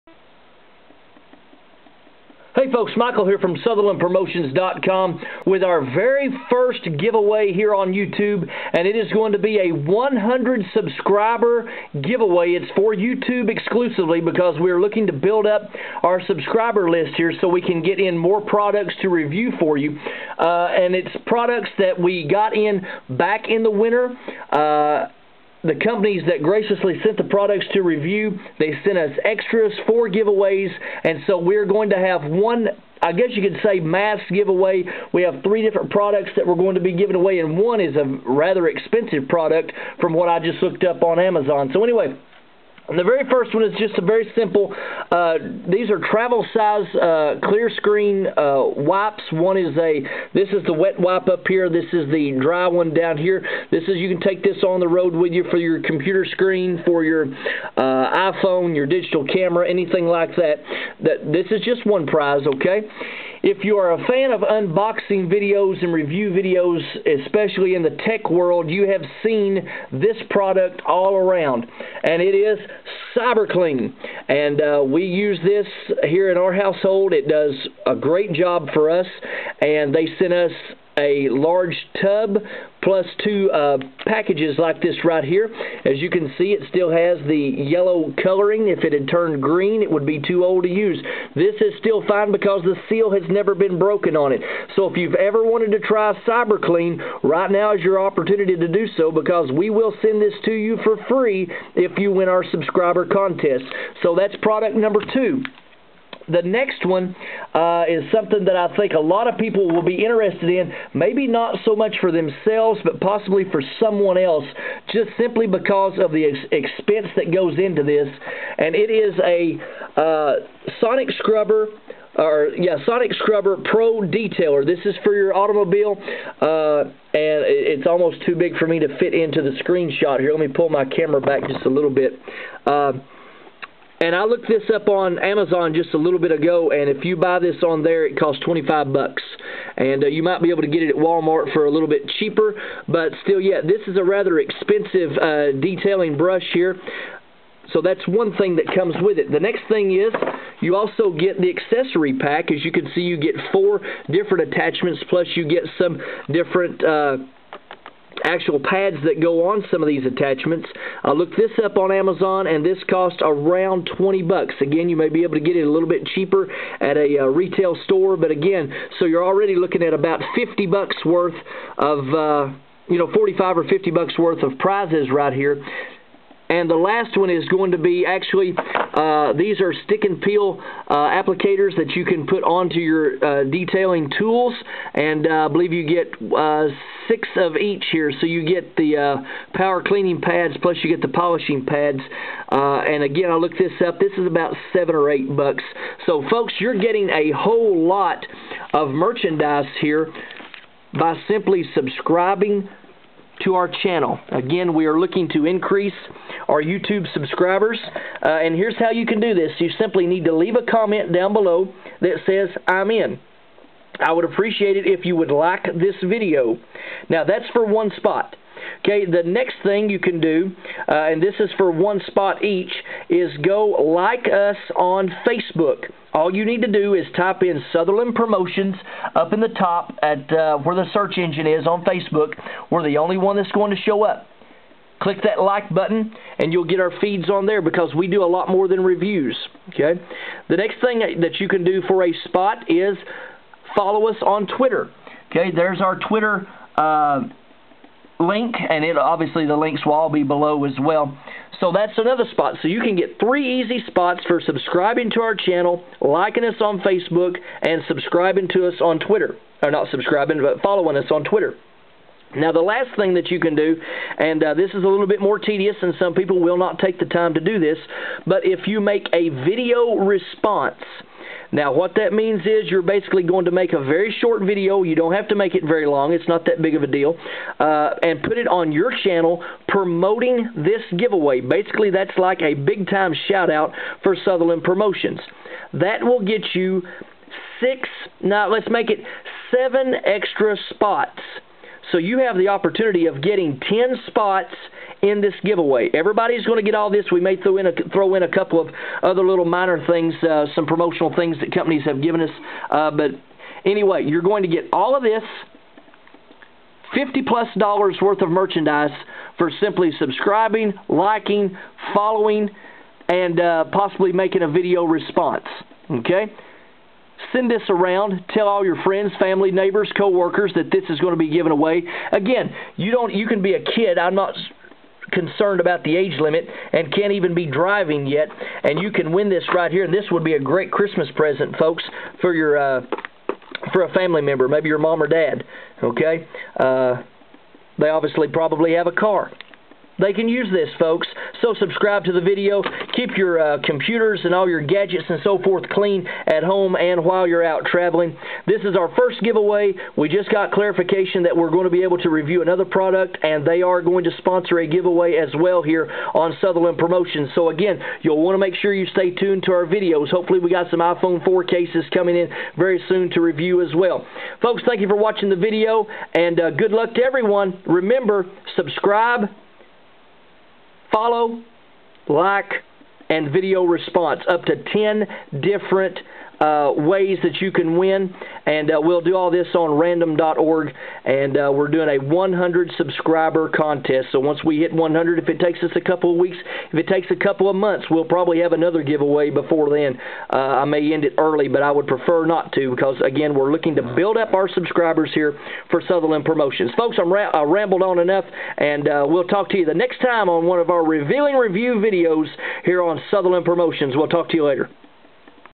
Our 1st 100 Subscriber Contest
Here are the ways you can earn spots to win over $50 worth of prizes:

Additional spots can be earned by:

Liking us on Facebook - simply search for " Southerland Promotions "  for one additional spot.

Please let us know in your comments below that you have "liked" on facebook & follow on twitter with your user names.

For 7 additional spots - make a video response promoting our channel & giveaway (a short 30 second video will be fine). Please include our YouTube link in your description

That's a possible 10 spots towards winning all of the great prizes in the video.